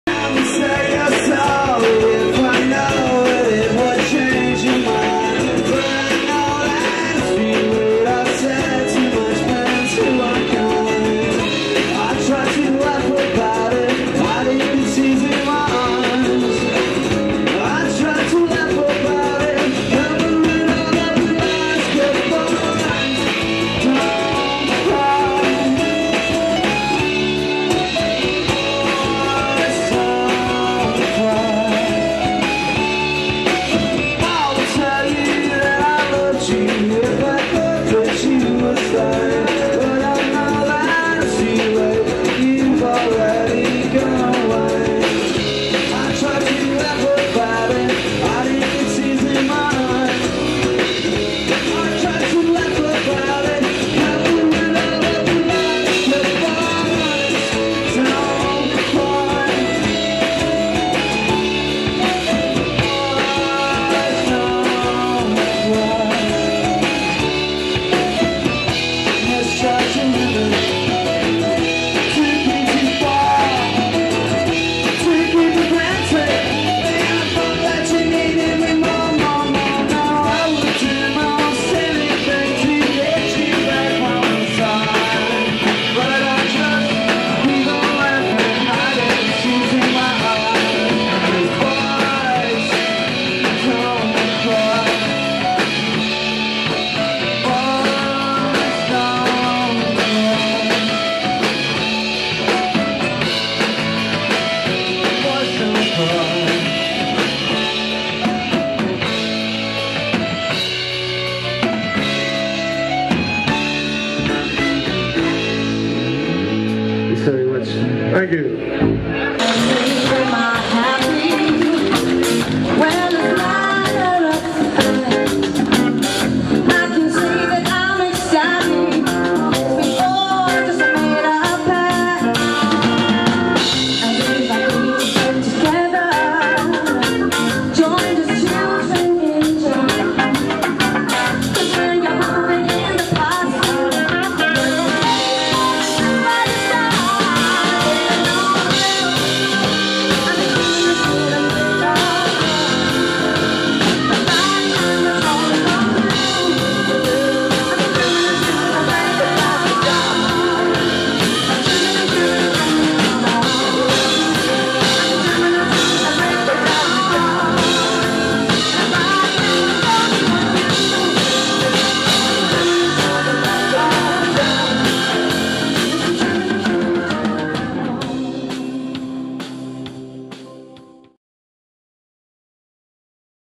Ard Ri Hotel 2004
The first band is The Jamtarts,the second band is Lady Mamouth with Aishling Brown !The year is 2004,opening party for Waterford Harley-Davidson.